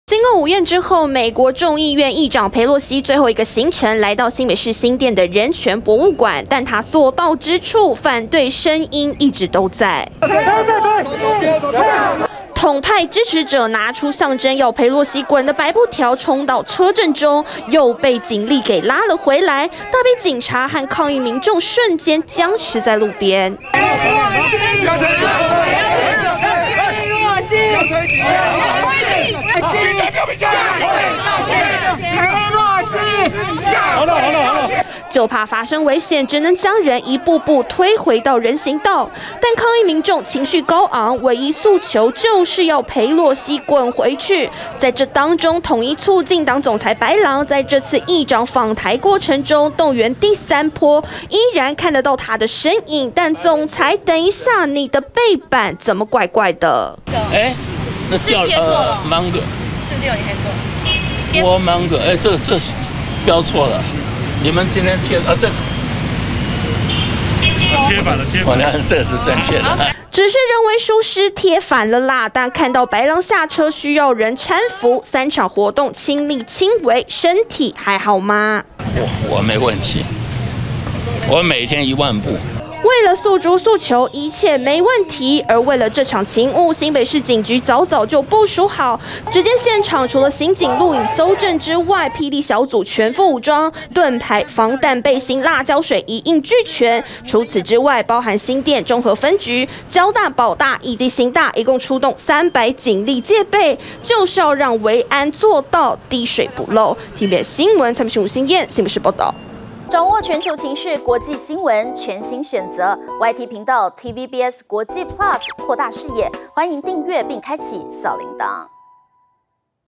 裴洛西訪人權博物館 統促黨三度動員抗議｜TVBS新聞
美國眾議院議長裴洛西最後一個行程，來到新北市新店的人權博物館，統派人士依然到現場舉牌、舉布條抗議，其中統促黨已經連續第三場動員，總裁白狼張安樂同樣現身；為了這場勤務，新北市警局早早就部署好，三百名警力現場戒備，就是要避免擦槍走火。